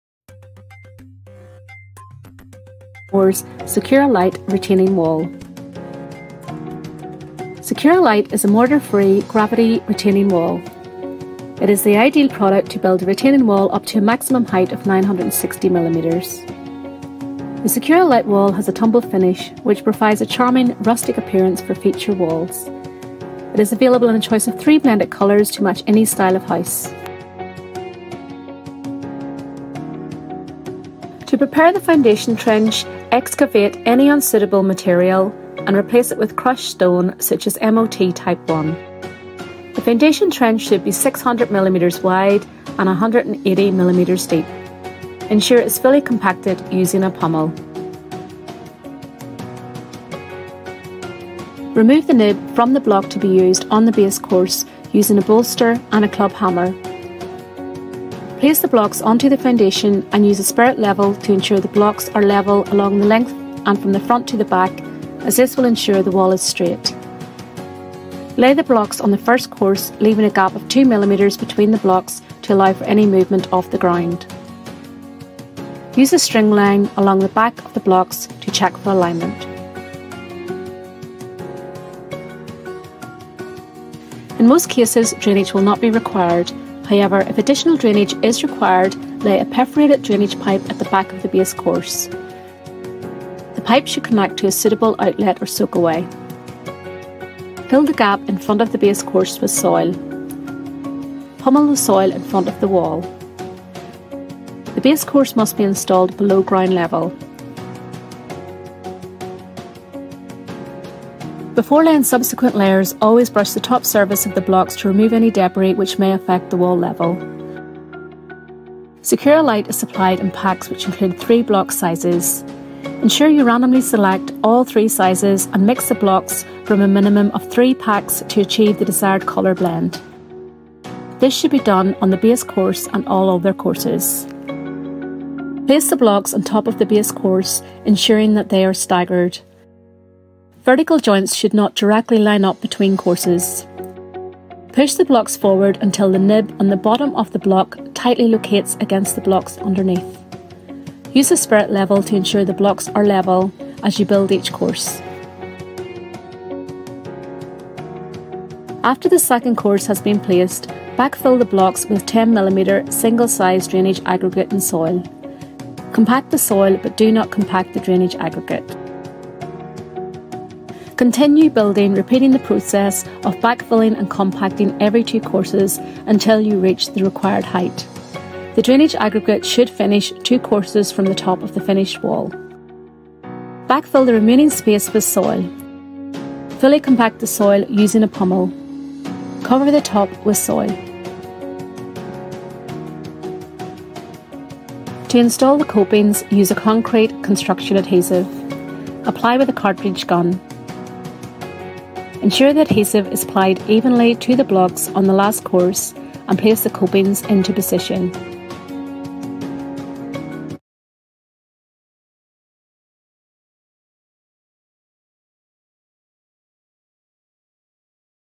Tobermore - How to Build a Secura Lite Retaining Wall
Check out this step-by-step guide on how to build a Secura Lite Retaining Wall.